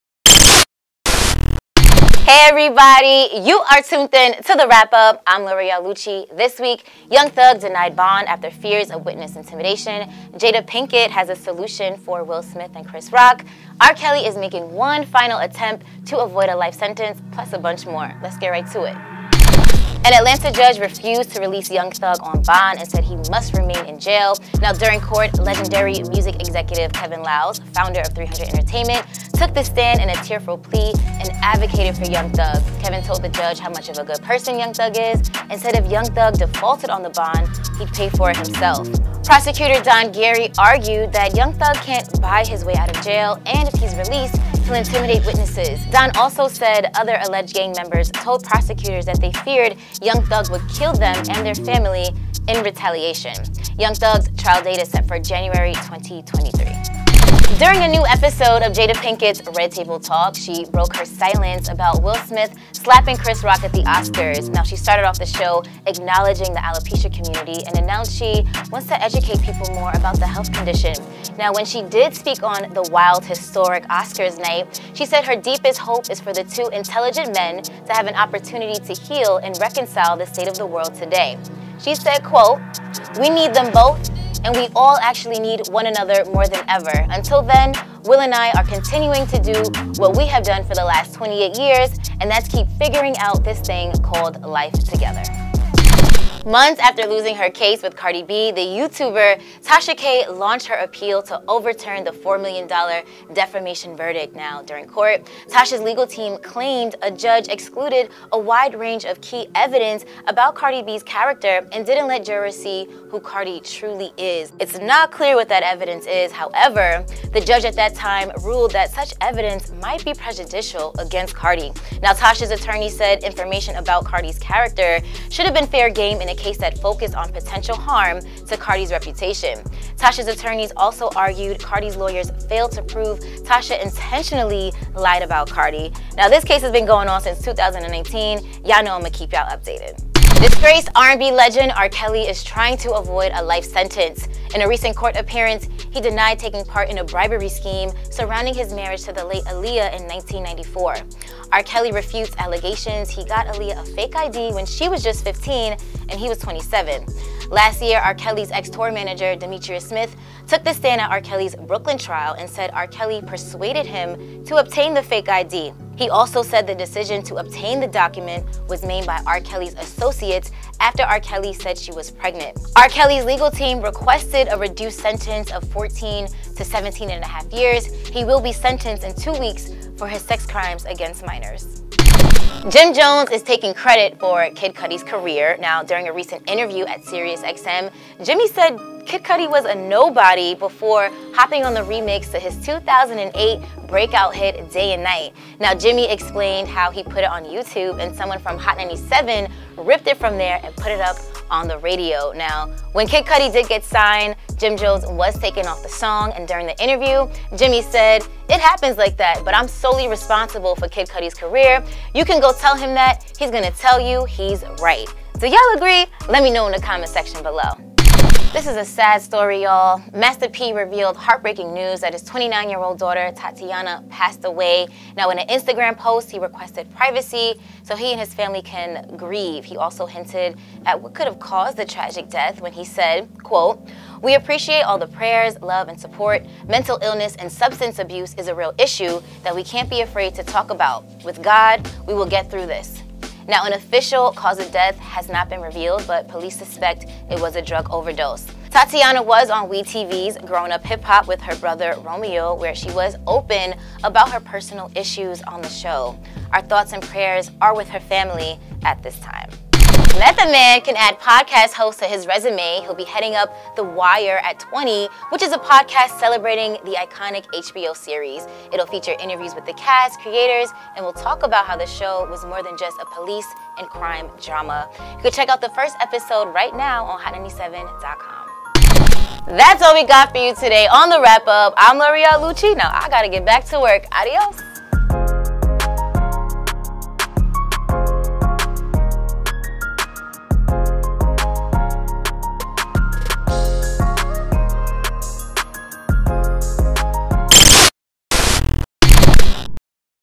R. Kelly's Last Shot To Beat Life Sentence, Jada's Solution for Chris Rock and Will Smith
R. Kelly makes a last ditch attempt to avoid a life sentence, Jada Pinkett's Solution for Chris Rock and Will Smith's "Healing", Young Thug Denied Bond after emotion plea from record exec Kevin Liles.